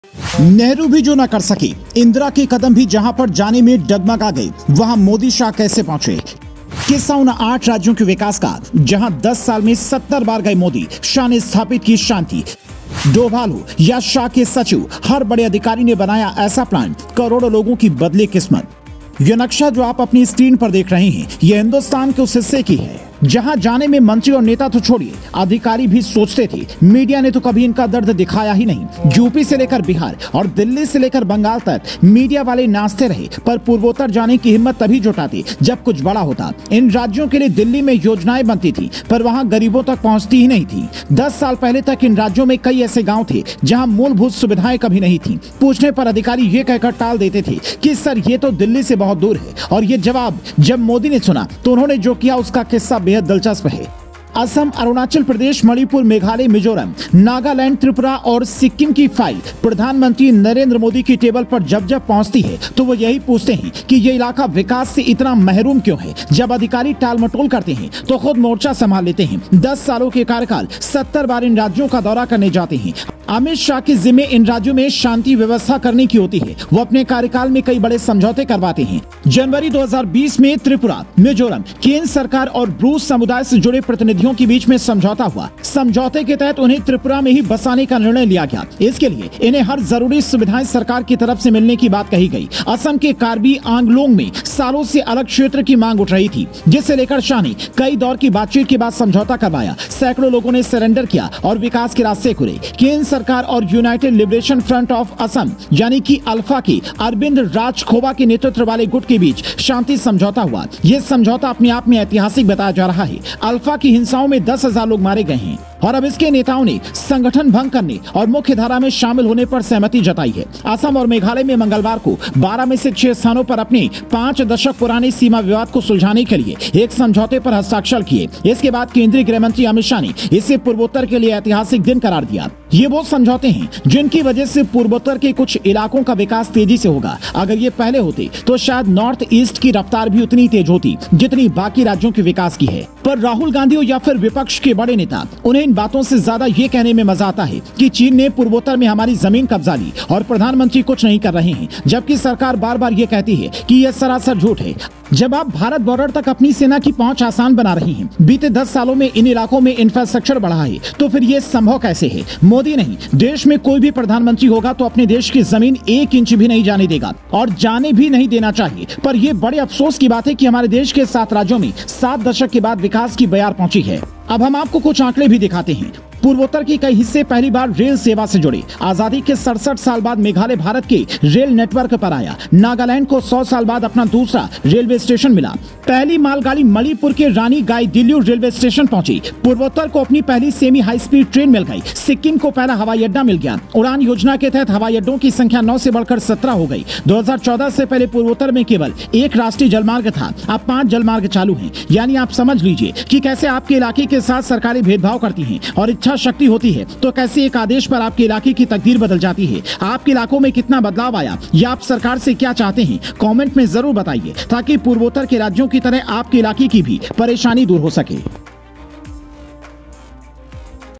जो कोई प्रधानमंत्री और गृहमंत्री नहीं कर पाया, वो मोदी-शाह ने किया, राहुल गांधी भी मानेंगे लोहा
loksabhaelection2024